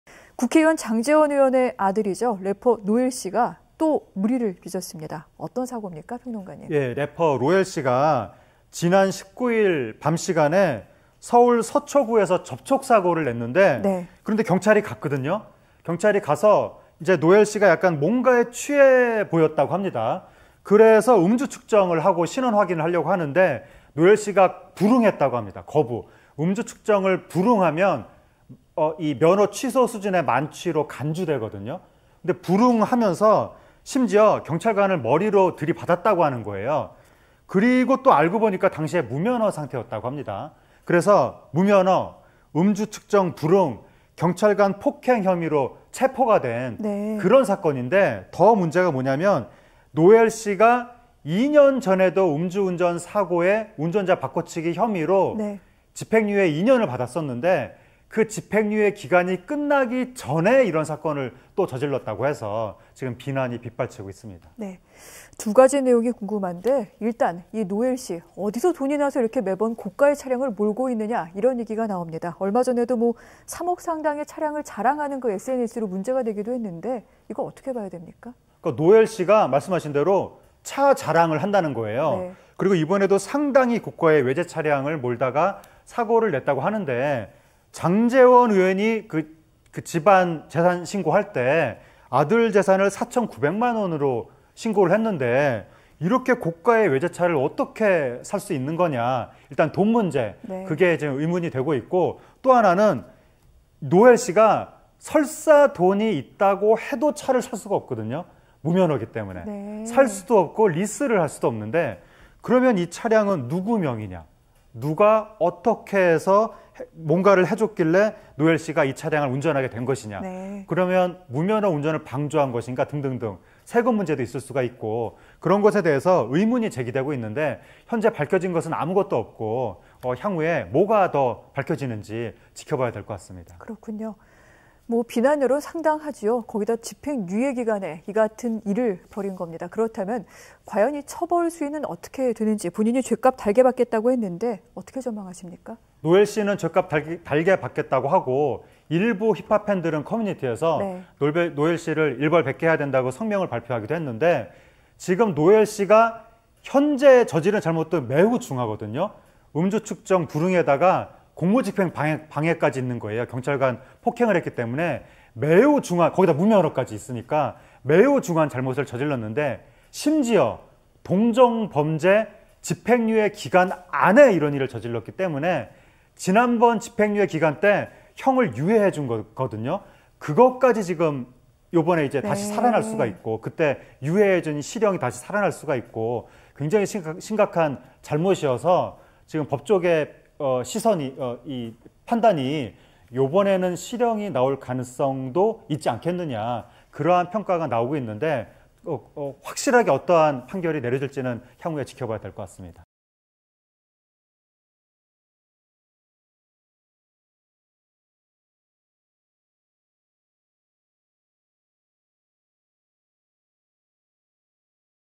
장제원 아들 노엘, 무면허 운전에 경찰 폭행..."집행유예 기간 동종범죄, 실형 가능성"
국회의원 장제원 의원의 아들
래퍼 노엘 씨가 또 다시 물의를 빚어 논란입니다.
무면허 운전도 모자라 경찰을 폭행한 것인데요.

더욱 논란이 된 부분은 집행유예 기간에
동종범죄를 저질렀다는 점입니다.
이 때문에 실형 가능성도 높게 점쳐지고 있는데요.
노엘은 어떤 판결을 받게 될까요?
간추린 오늘에서 살펴보겠습니다.